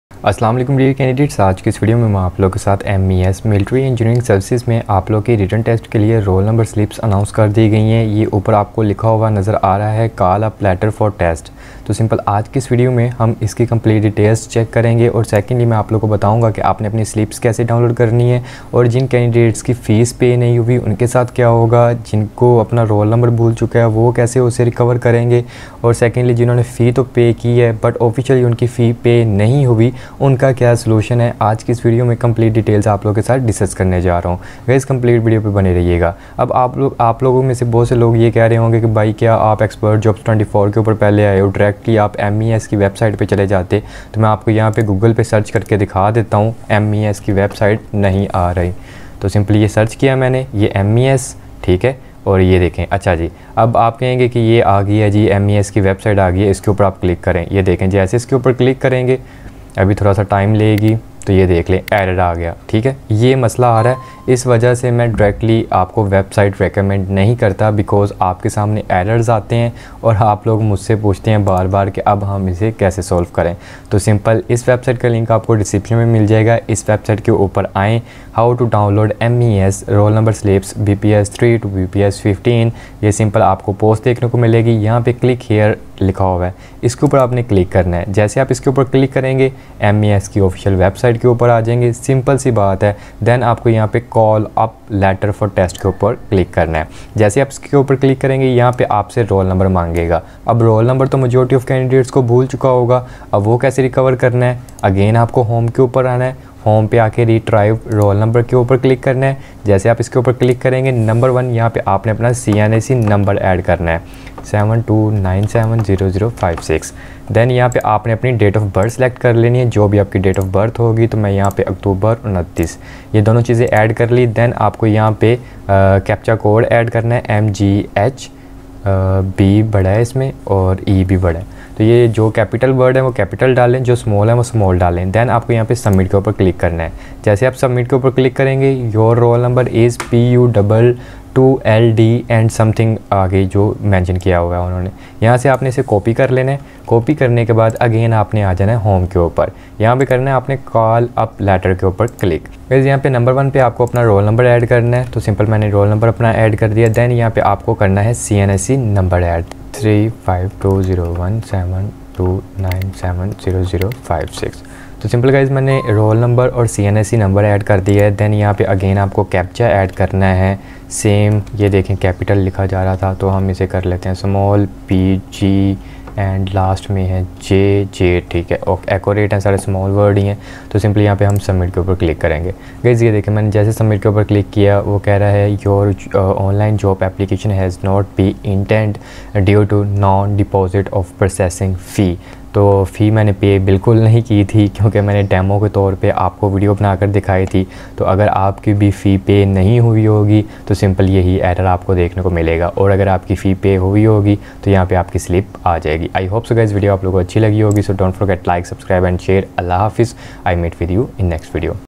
How to Download MES Written Test Roll Number Slips Bps 3-15 ايم وايس رول نمرسيلپ ڈاؤن لوڈ کريں
How to Download MES Roll Number Slips Bps 3-15 ايم وايس رول نمرسيلپ ڈاؤن لوڈ کريں